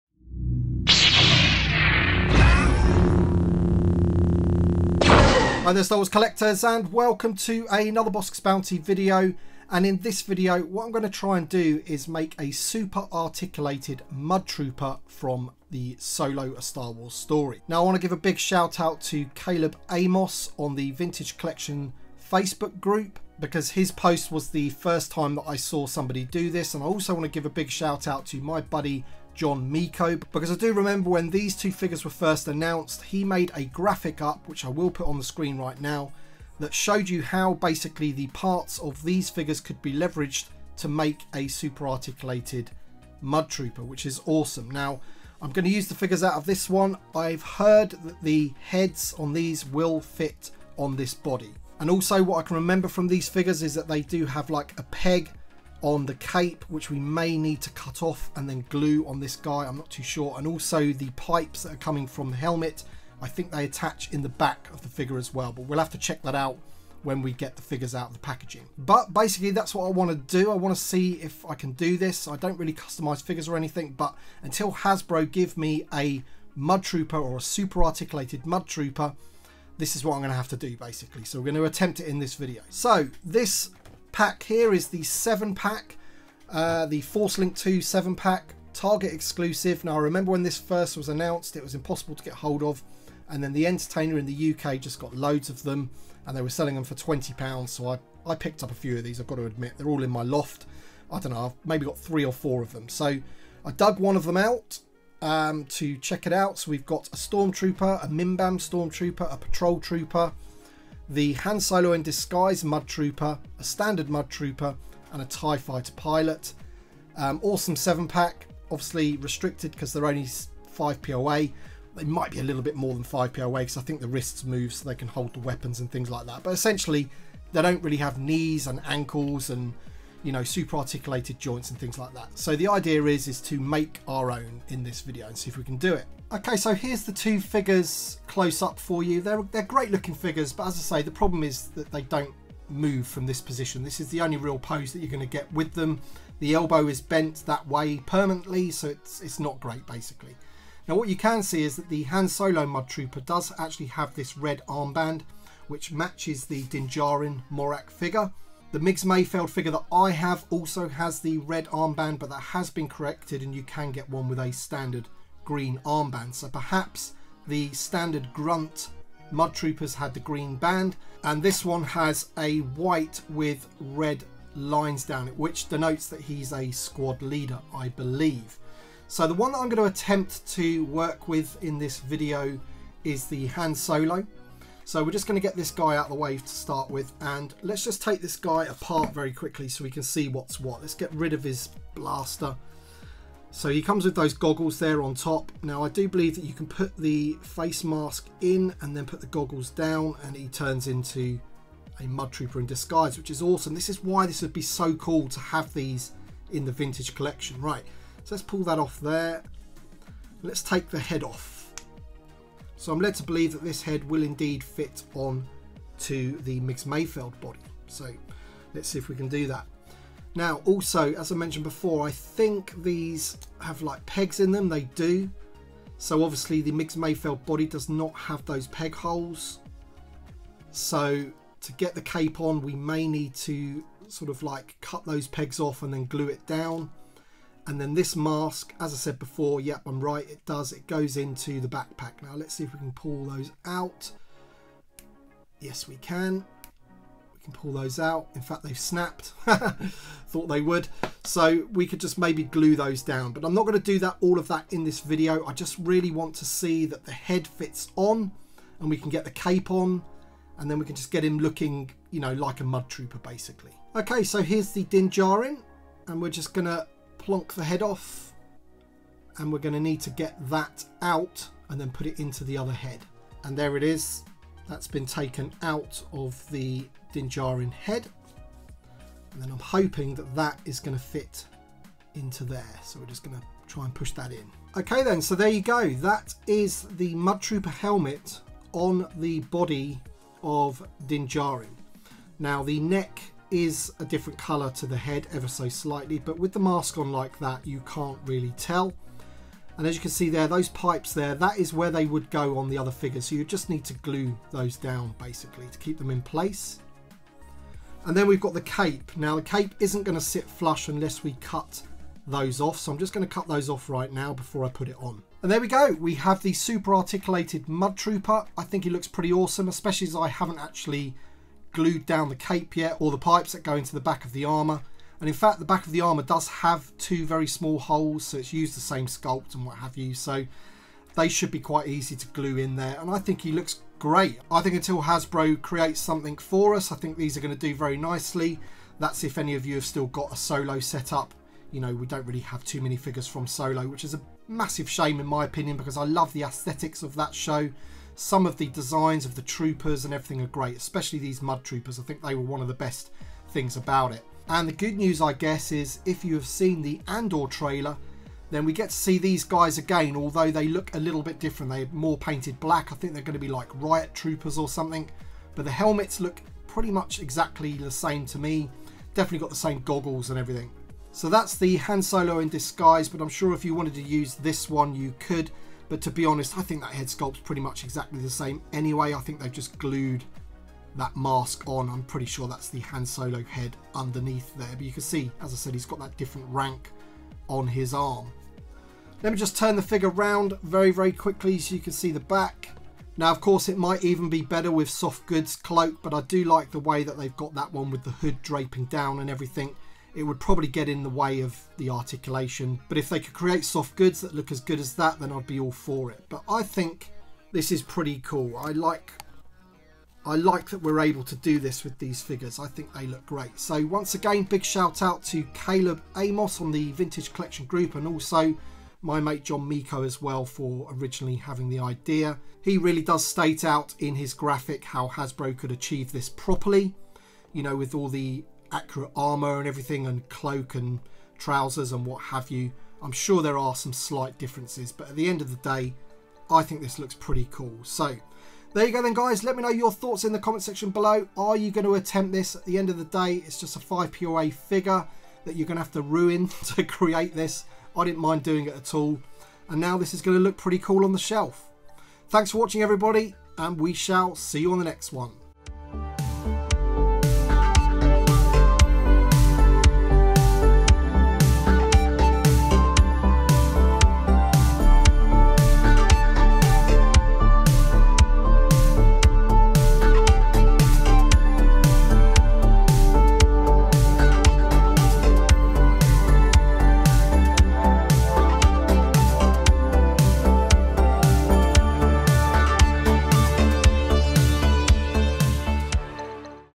Star Wars The Vintage Collection MUDTROOPER Custom!
Hi Star Wars collectors! In this video I take inspiration from Caleb Amos and John Miko from The Vintage Collection Facebook group and try too create my own custom super articulated Mudtrooper from Solo: A Star Wars Story.

Want to send some FAN MAIL? address below:
Bossk's Bounty
Worthing
BN12 9EW

I am a member of this awesome Star Wars Discord